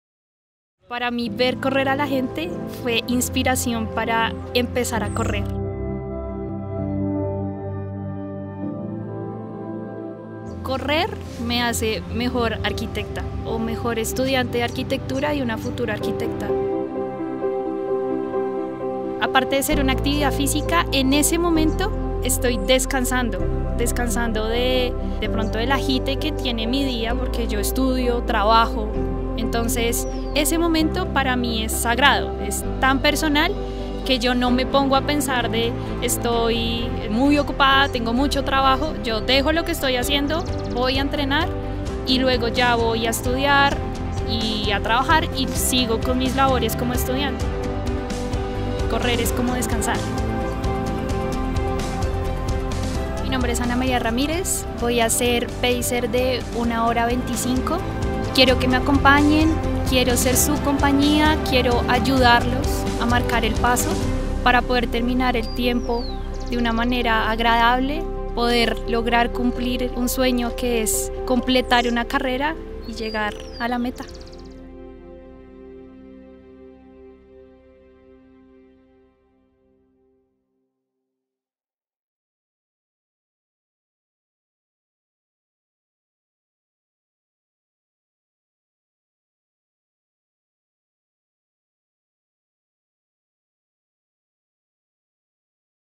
Un respiro. Ana María Ramírez. Pacer Carrera Allianz 15K.
"Correr es como descansar", Ana María Ramírez, pacer en la Carrera Allianz 15K, tiempo 01:25:00.
Valor de la inscripción $35.000.